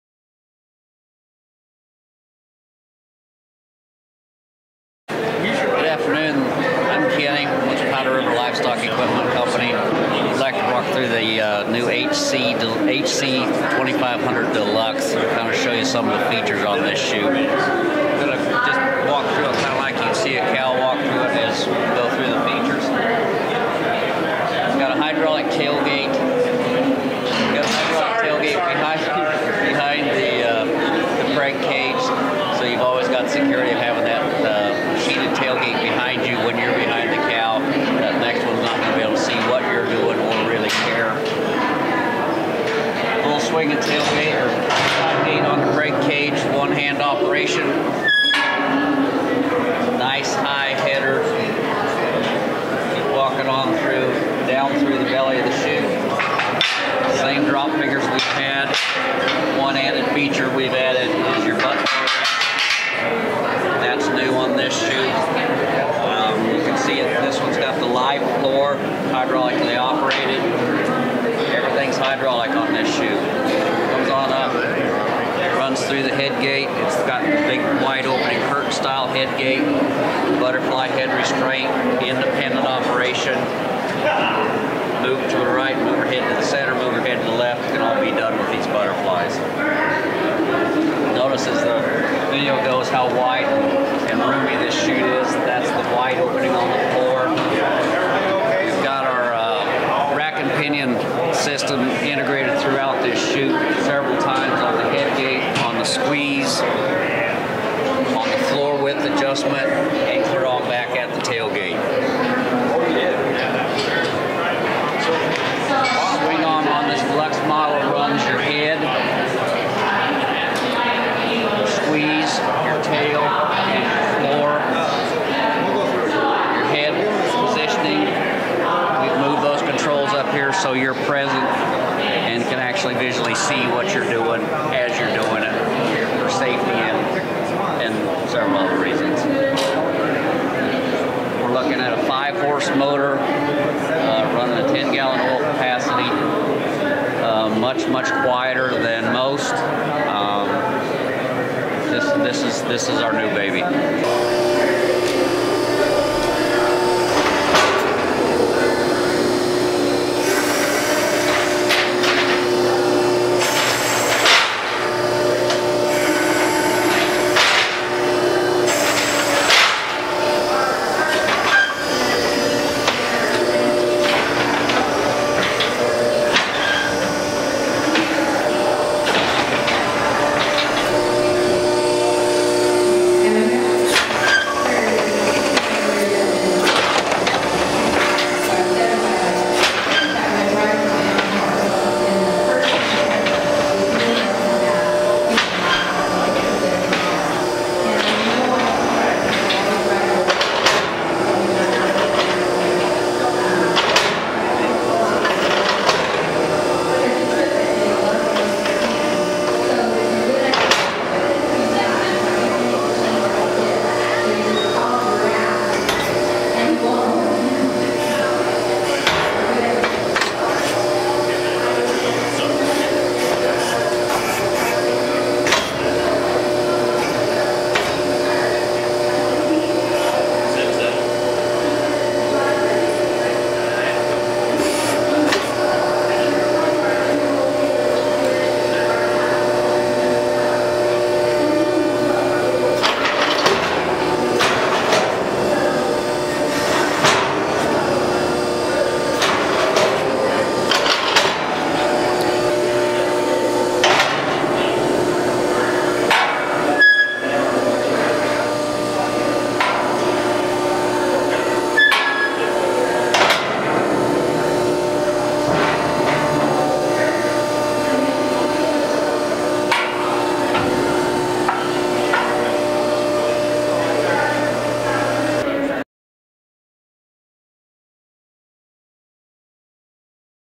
Tradeshow Walk Through of the HC2500 Deluxe Hydraulic Chute
Kenny talks through the HC2500 Deluxe chute that was on display at the National Cattlemen's show in Nashville, KY February 2017.

The Audio is a little tough to hear because of the background noise of the show.